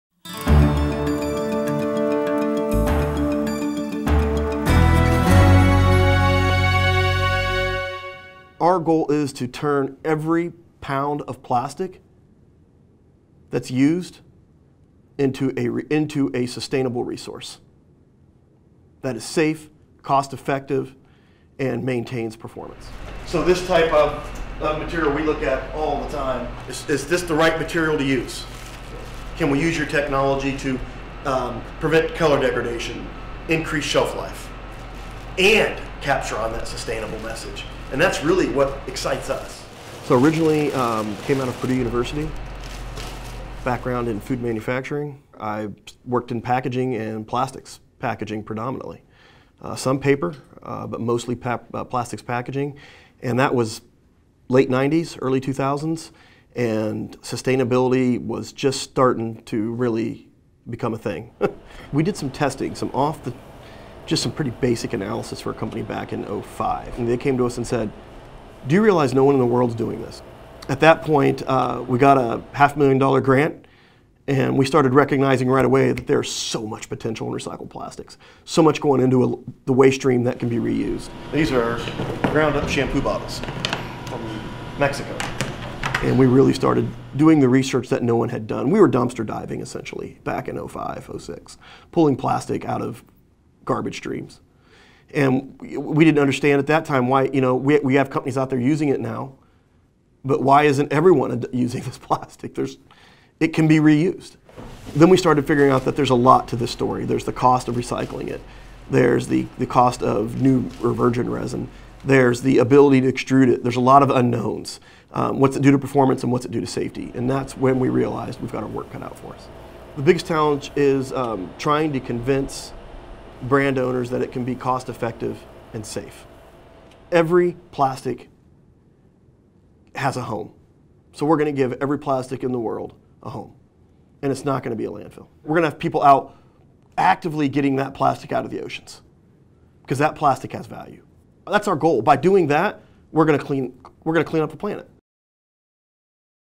Change Agent - Keith Vorst
Keith Vorst wants to clean up the planet. An associate professor of food science and human nutrition at Iowa State University, Vorst has been studying ways to increase recycling efforts, and improve the performance of plastic packaging for years. His research has led to dozens of patents, and Vorst and his team have worked with some of the largest companies in the world. His goal is to make plastic into a valuable, sustainable resource that leads to less waste and a cleaner environment.